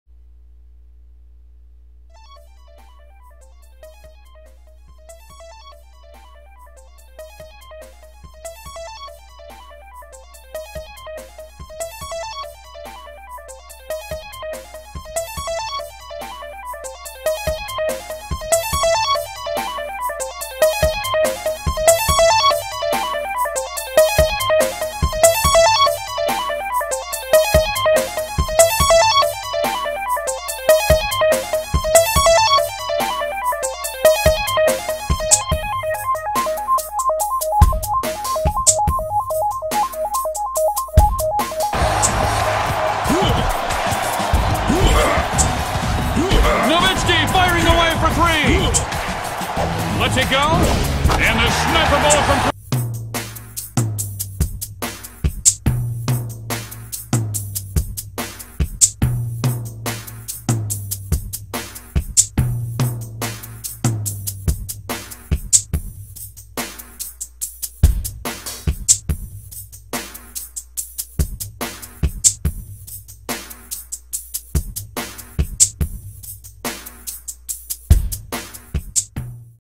NBA Jam: On Fire Edition Review (XBLA)
NBA Jam for the arcade was one of the all time great video games. Check out the review of the new revamped NBA Jam: On Fire Edition. You won't be disappointed.